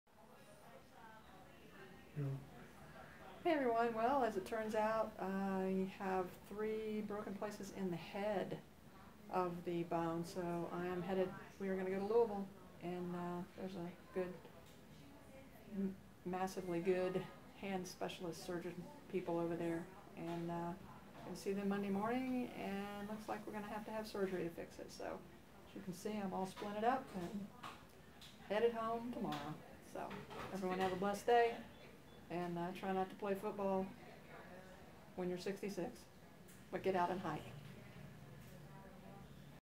Needs surgery to fix it, going to Louisville tomorrow
Update broken wrist